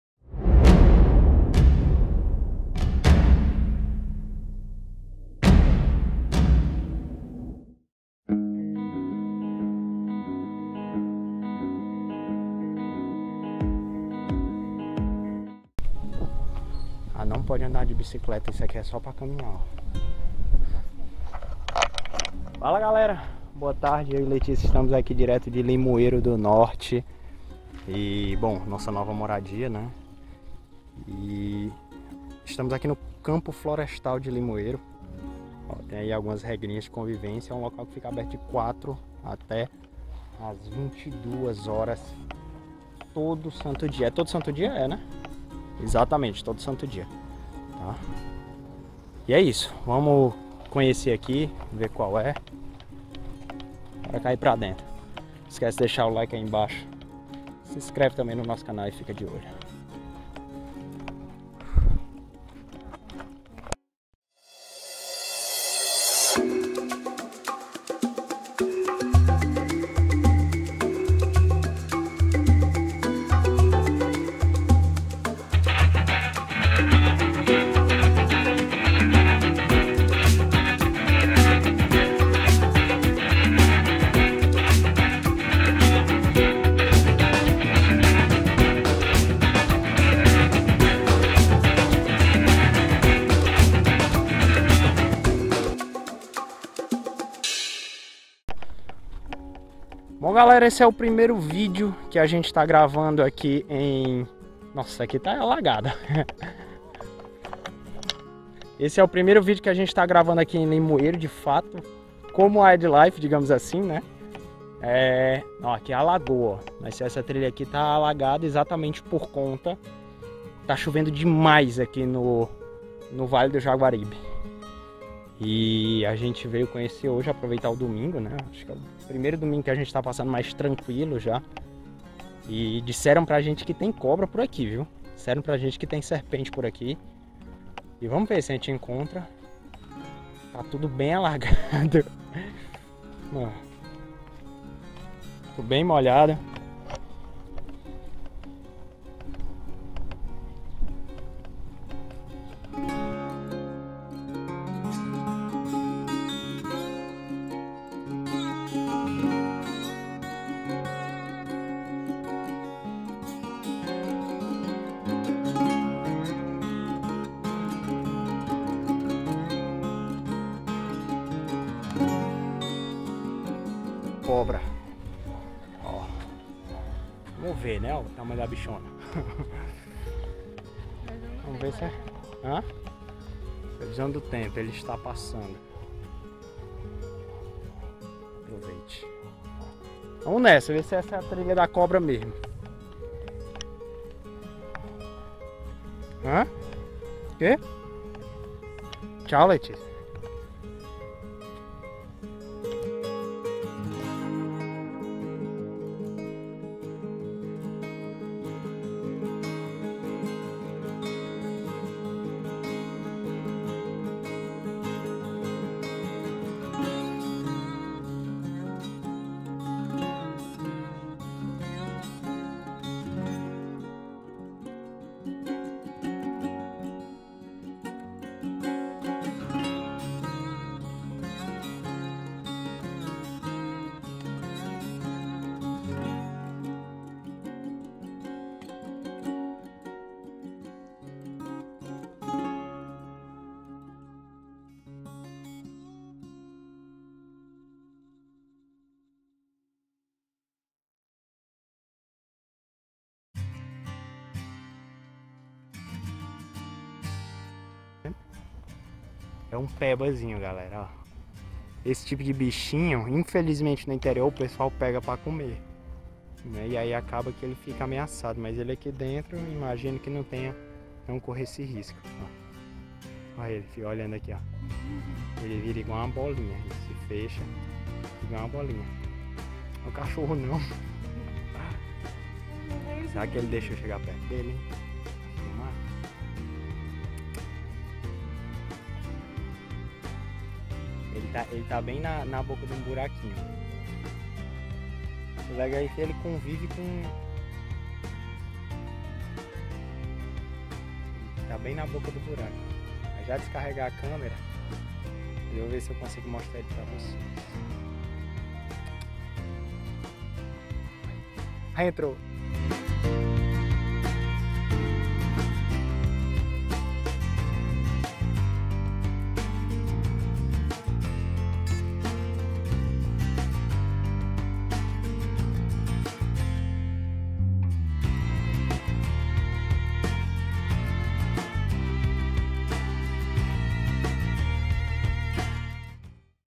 Conhecendo o Campo Florestal de Limoeiro do Norte/CE
Nossos aventureiros Tiago e Letícia foram conhecer a área verde municipal de Limoeiro do Norte, nova casa deles no Vale do Jaguaribe.
Endereços para envios de equipamentos, etc:
WildLife Ceará
Rua Francois Teles de Menezes, n. 188, Ap. 1208, Fátima.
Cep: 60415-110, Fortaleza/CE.

Vamos pra natureza!